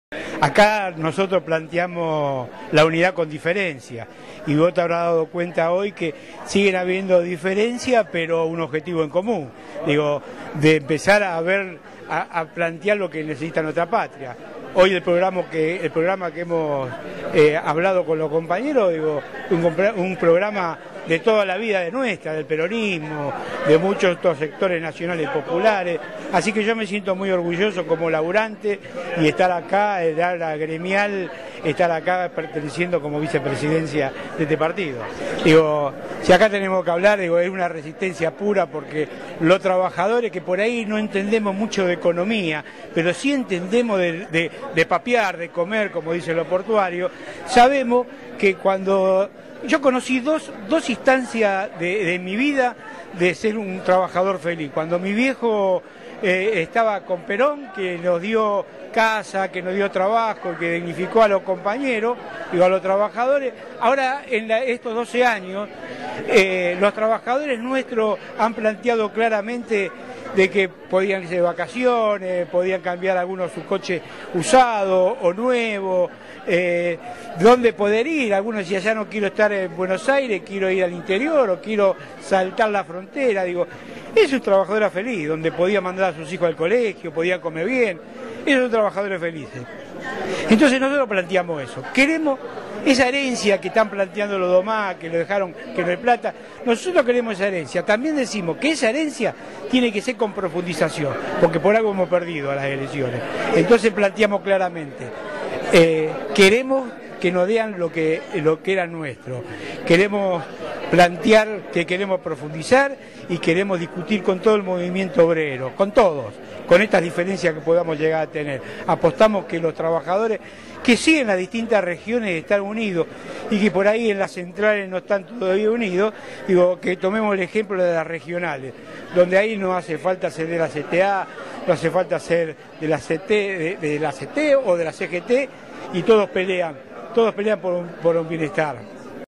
CUANDO LAS DIFERENCIAS ENRIQUECEN LA UNIDAD
Adolfo Barja, Vicepresidente de Miles 3T explica a CN10TV San Rafael (único medio mendocino presente en el encuentro realizado en Capital Federal) los objetivos del Frente Miles 3TV.